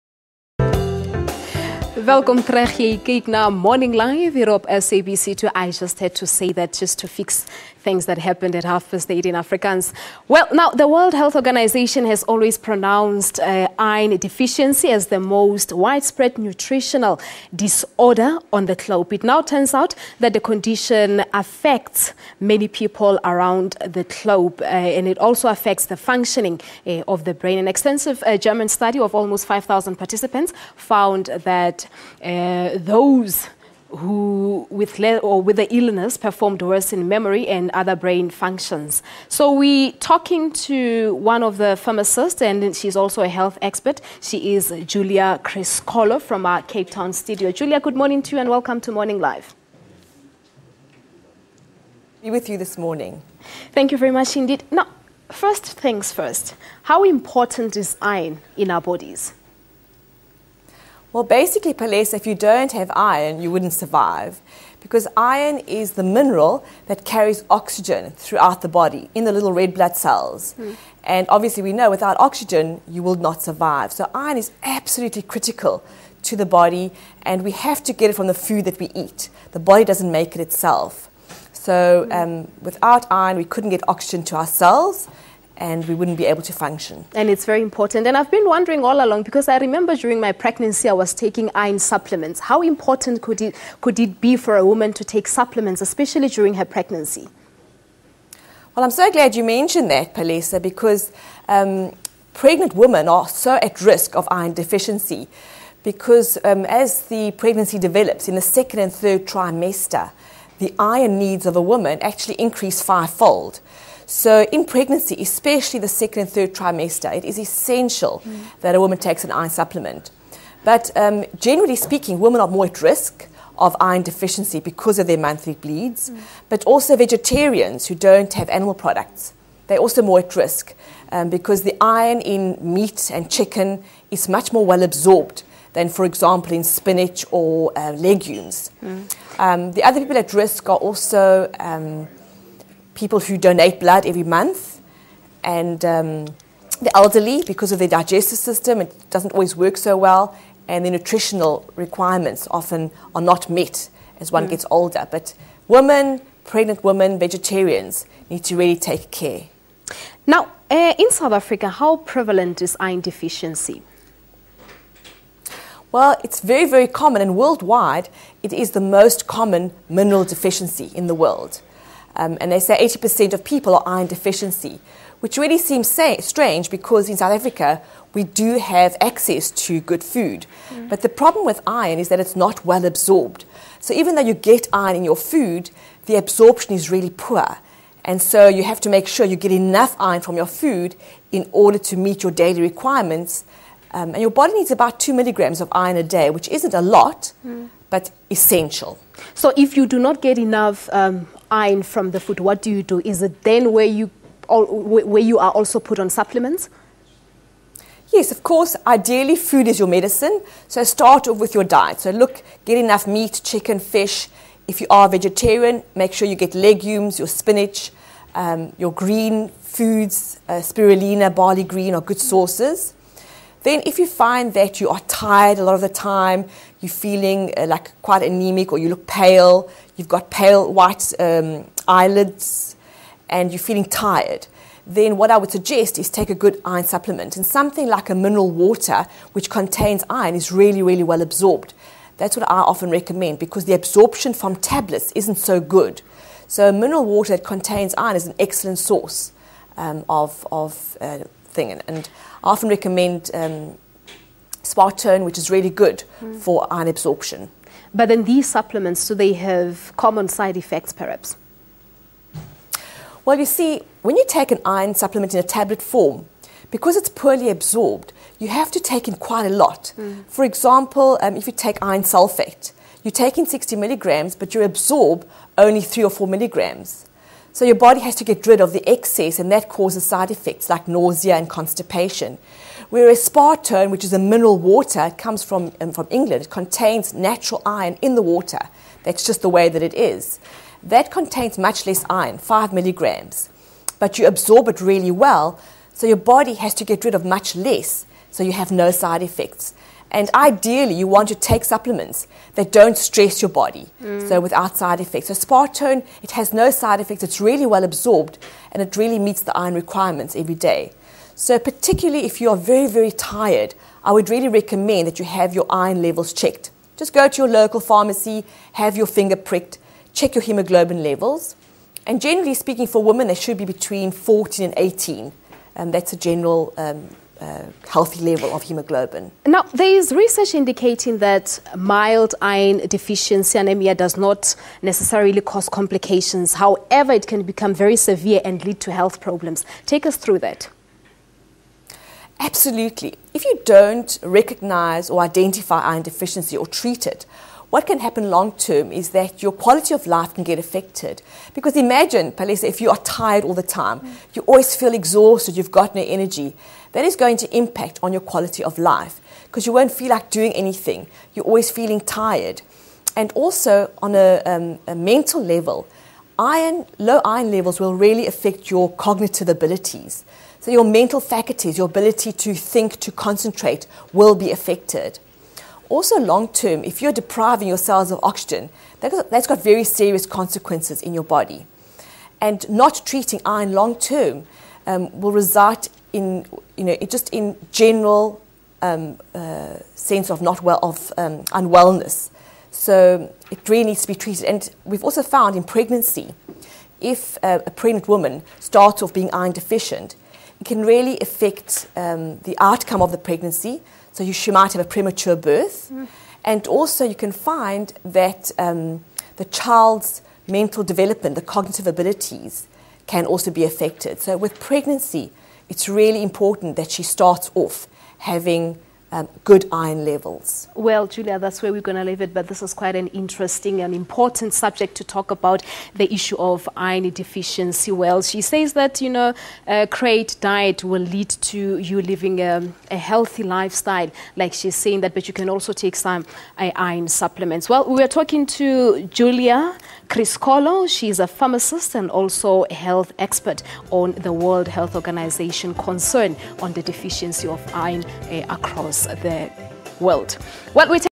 Giulia Criscuolo on Iron deficiency
The World Health Organisation has always pronounced iron deficiency as the most widespread nutritional disorder on the globe. It now turns out that the condition affects the functioning of the brain. An extensive German study of almost five thousand participants found that those with the illness performed worse in memory and other brain functions than non-anaemic subjects. To tell us more we are  joined by Pharmacist and  health expert Giulia Criscuolo.